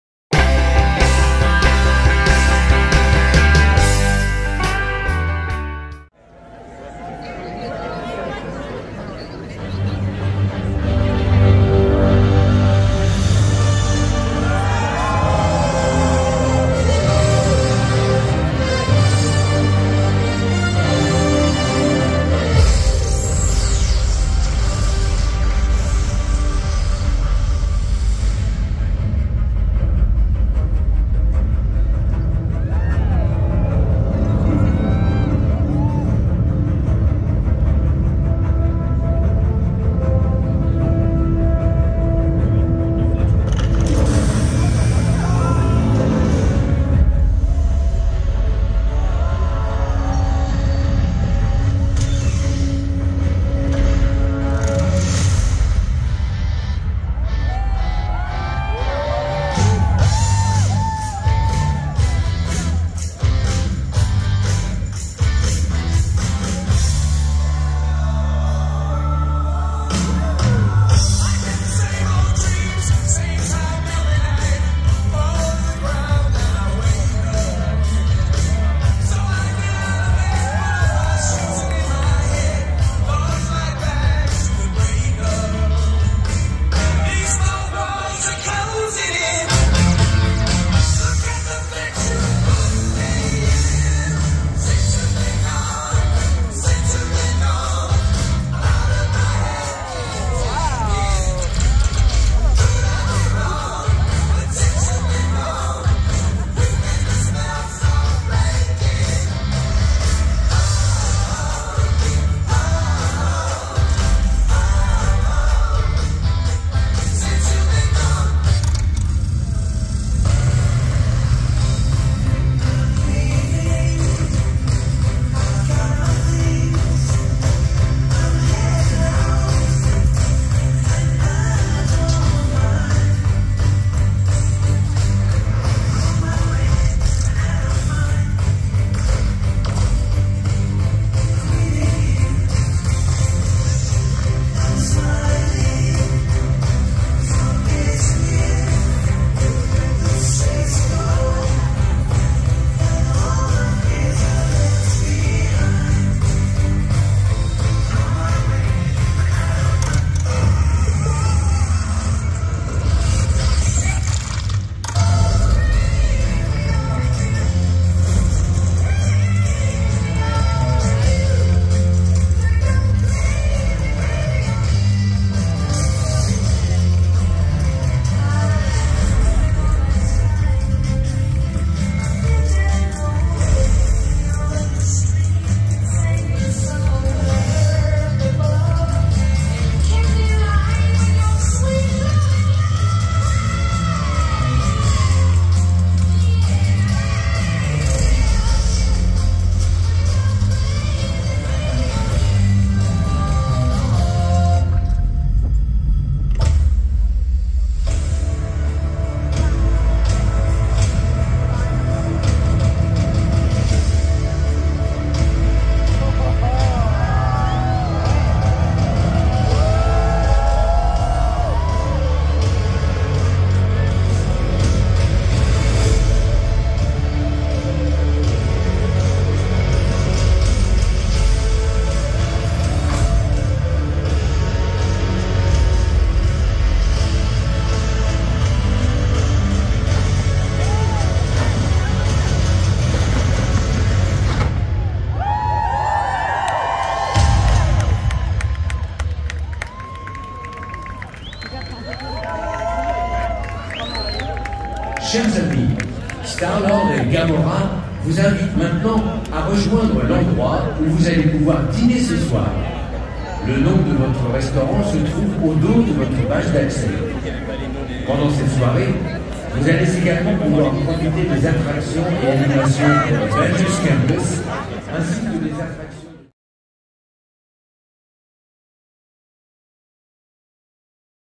Show du Gala Gardiens de la Galaxie vol 3 du 22 Avril 2023
Spectacle du Gala Gardiens de la Galaxie vol 3 pour  la promotion du film au parc Walt Disney Studio du 22 Avril 2023